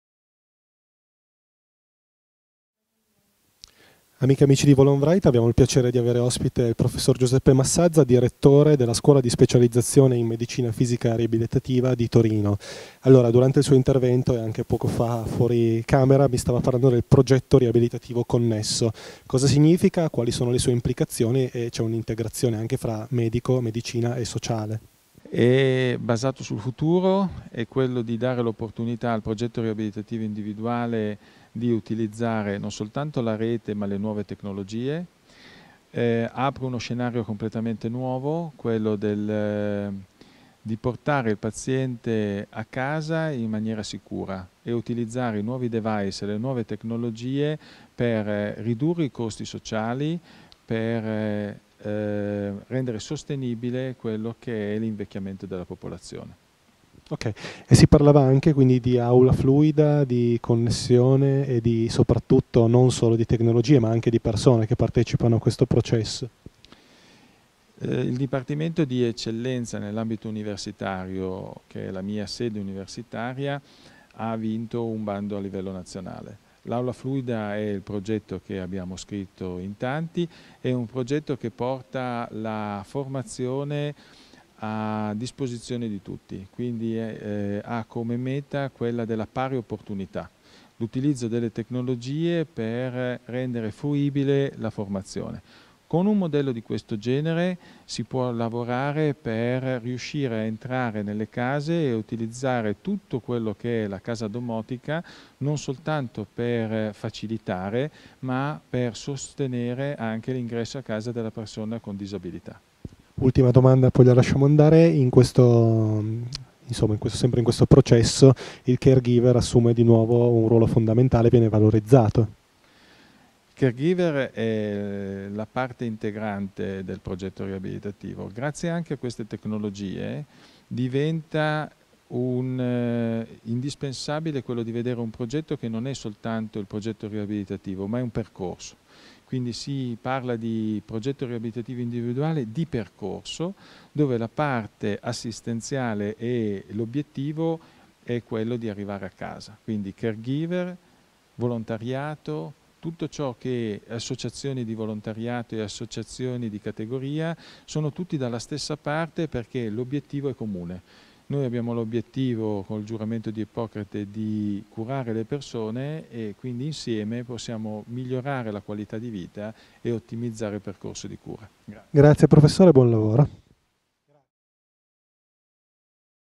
INTERVISTA A GIUSEPPE MASSAZZA
Il 3 Dicembre abbiamo partecipato al Convegno "La Medicina Fisica e Riabilitativa: 60 anni di impegno per la cura delle persone con disabilità" in occasione del 60° anniversario della SIMFER - Società Italiana di Medicina Fisica e Ribilitativa.
Qui l'intervista a Giuseppe Massazza, Direttore della Scuola di Specializzazione in MFR di Torino